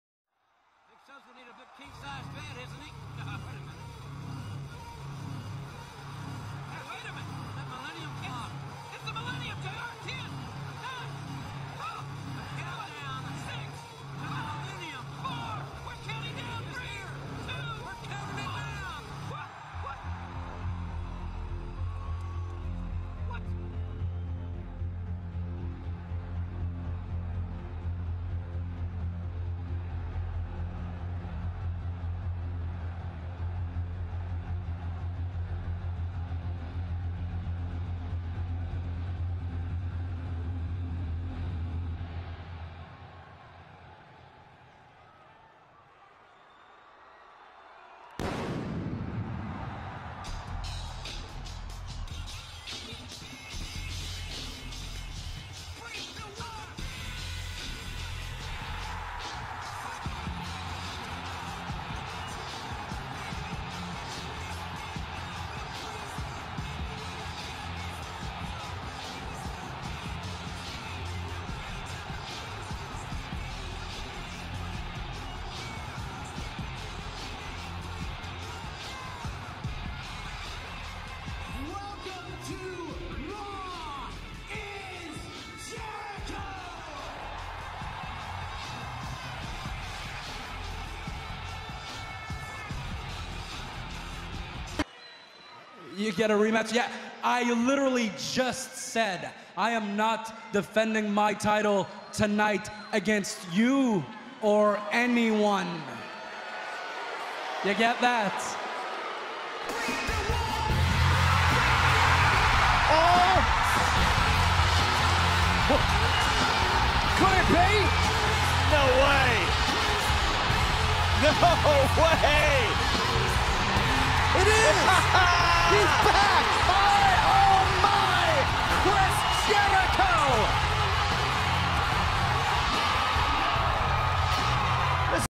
Chris Jericho's first & last entrances in the WWE - THE GOAT
Cchris jericho's first and last entrances in the WWE - It's THE GOAT's entrances ,just enjoy it MANNNNN.

Hope that was not the last time we saw CHRIS JERICHO in the WWE